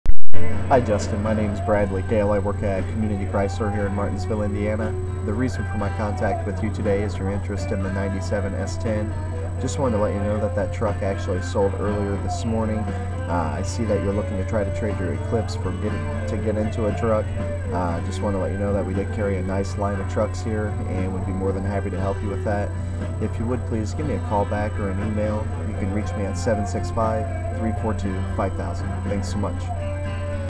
Video for Justin | Brad Kale | Community Chrysler Martinsville
Justin's first video response from Brad at Community Chrysler